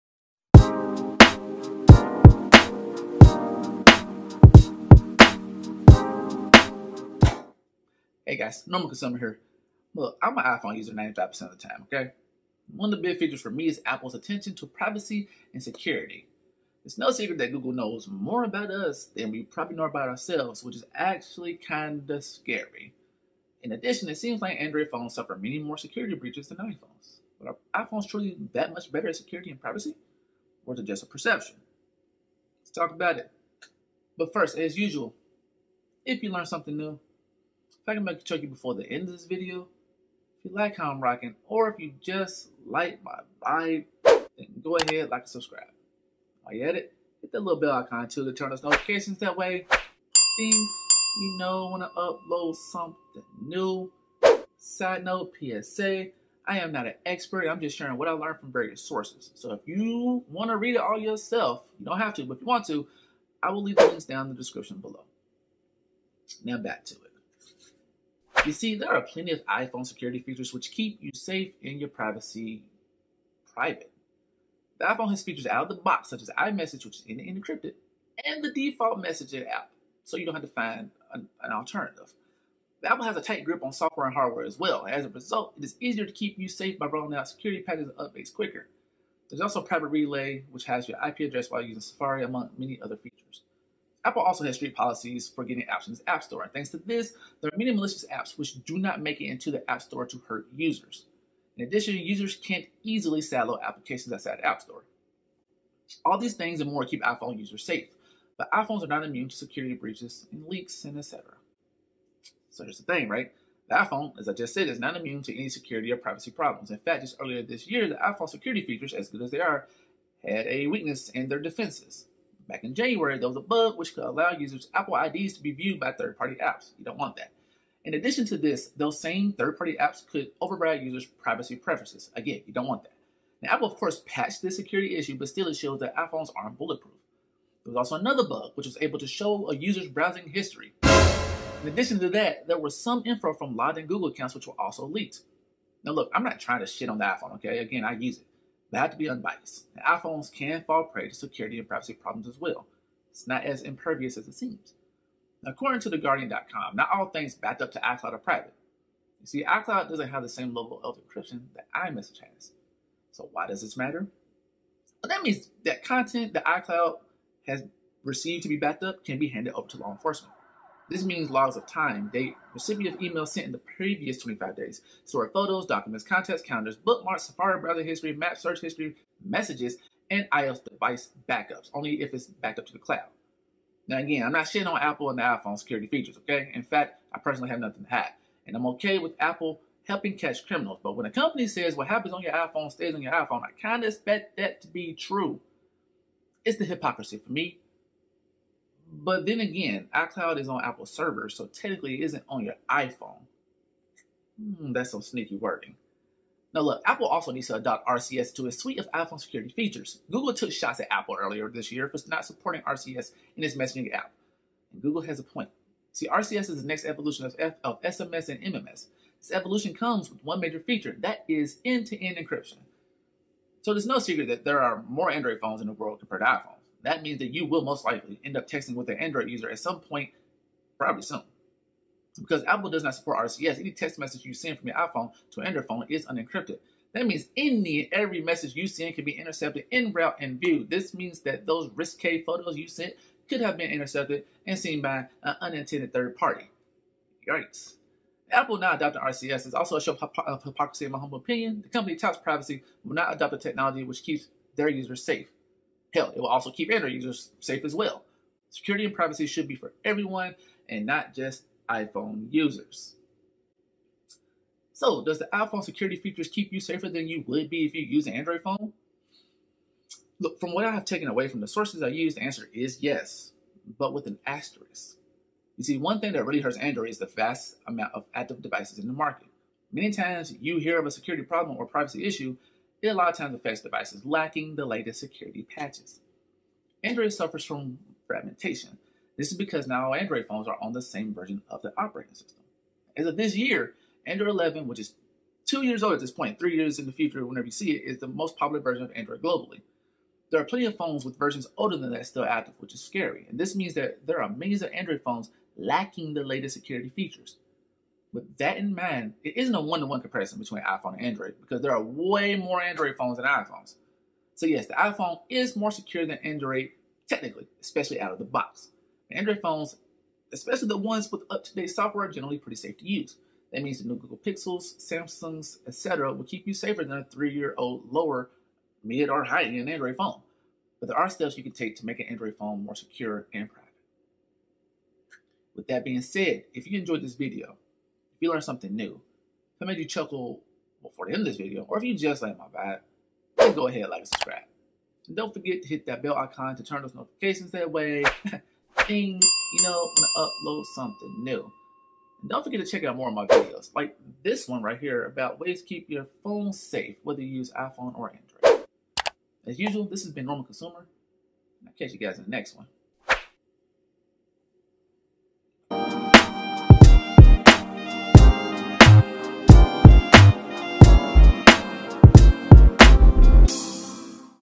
Is iPhone privacy really impenetrable?
One of Apple's main selling points is iPhone privacy are more private than Android phones. While this is true in some cases, iPhones aren't perfect.

[CHAPTERS]
00:00 Introduction
01:07 A Few iPhone Security Features
01:54 Two Recent Privacy Mishaps
02:35 Law Enforcement Can Easily Get Your iCloud Content
03:25 Google Was Right, Apple Needs RCS
04:25 Wrap Up: Comments And Thoughts